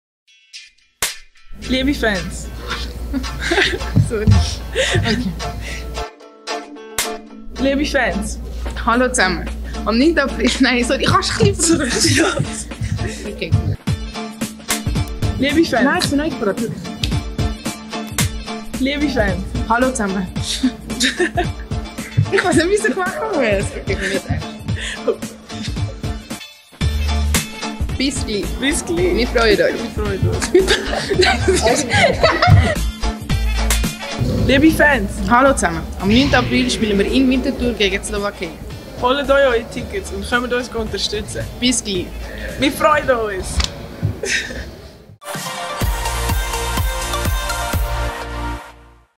Outtakes Aigbogun & Sow
Eseosa Aigbogun und Coumba Sow hatten viel Spass beim Videodreh; dabei klappte nicht alles auf Anhieb ;-)